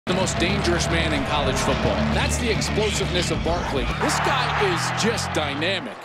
Saquon Barkley - Season Stats
Penn State running back Saquon Barkley totaled 172 all-purpose yards, highlighted by a 97-yard kickoff return and a 36-yard touchdown run at Ohio State. His season stats through eight games.